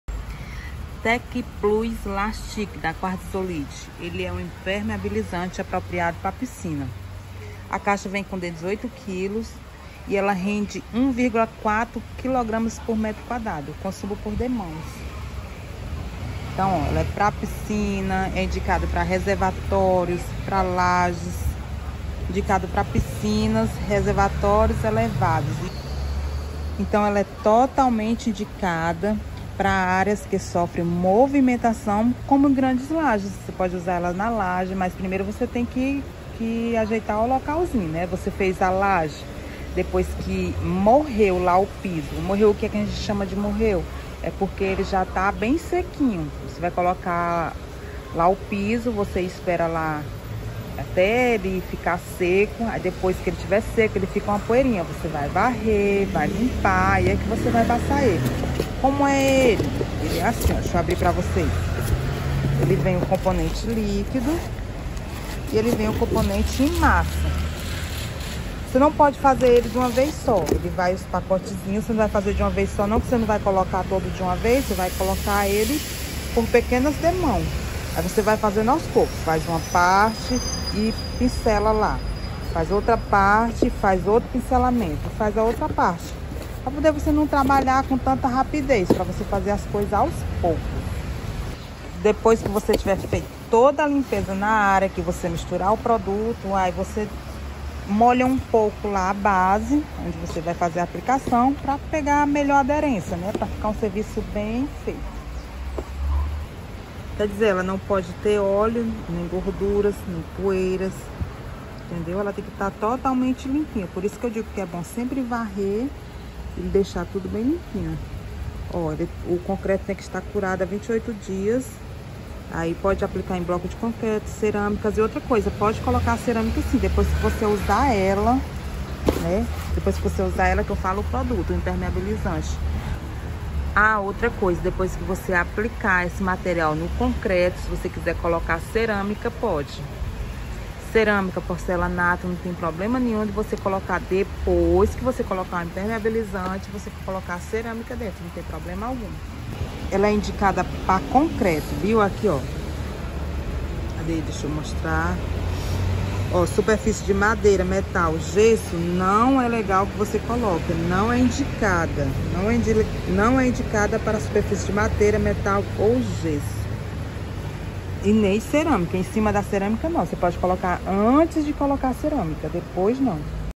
Tecplus lástic quartzolit impermeabilizante indicado para área interna e externa e piscina
Esse é um impermeabilizante indicado para lajes,terraço e sacada,reservatório de água piscina fria e aquecida pisos e paredes